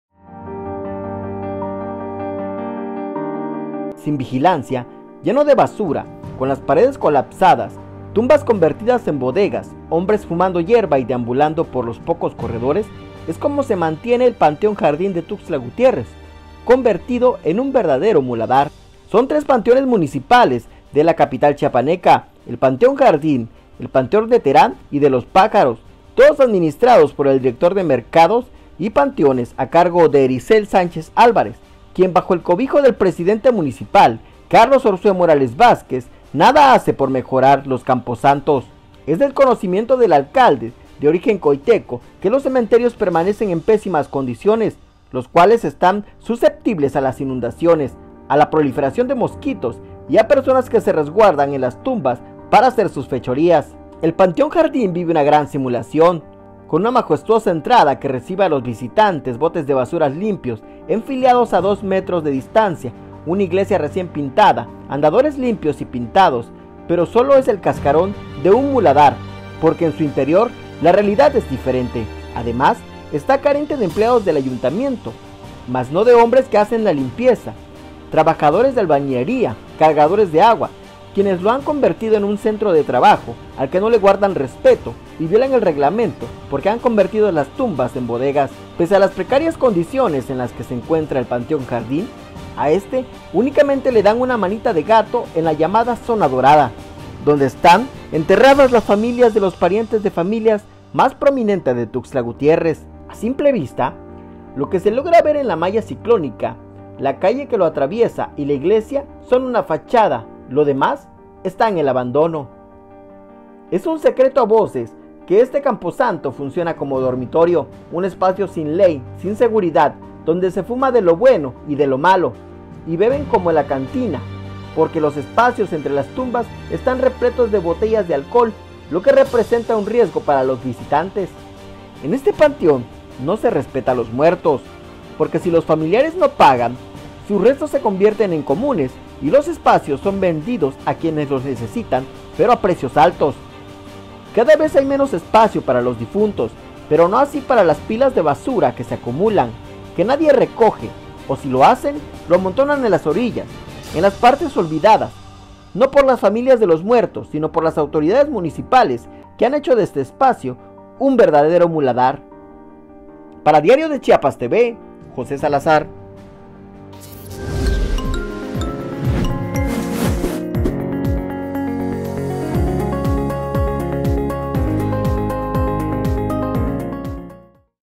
Panteón Jardín, un muladar
⚫⚰️ Videorreportaje | Sin vigilancia, lleno de basura, con las paredes colapsadas, tumbas convertidas en bodegas, hombres fumando hierba y deambulando por los pocos corredores, es como se mantiene el Panteón Jardín de Tuxtla Gutiérrez, convertido en un verdadero muladar

Entérate de todos los detalles en un reportaje de José Salazar. | Servicios informativos del